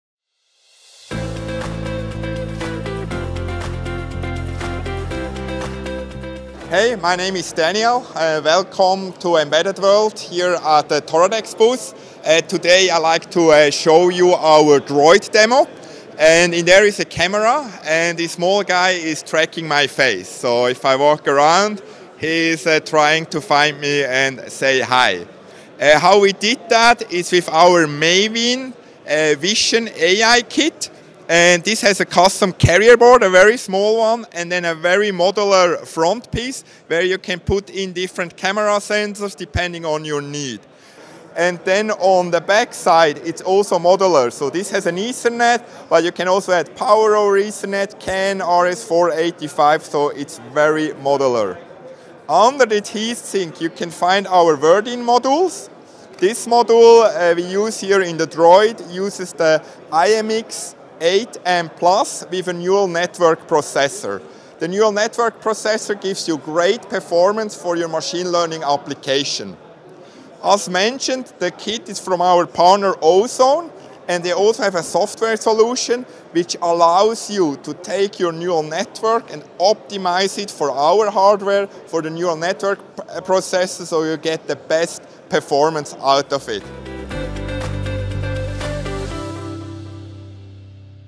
Edge AI, Machine Learning Head Pose Droid - Au-Zone Modular AI Vision Kit, NXP i.MX 8M Plus | EW2023
Join Daniel Lang, our CMO, and this friendly Real-time Head Pose Estimation droid at the Toradex booth at Embedded World 2023!

This little guy uses our partner Au-Zone's Modular AI Vision Kit, Maivin, with a custom carrier board and our Verdin iMX8M Plus SoM and neural network processor - based on the NXP i.MX8M Plus Application Processor - ideal for high performance and accuracy Edge AI and Machine Learning projects.

It also includes Au-Zone's software solution, which allows you to optimize the hardware and software integration!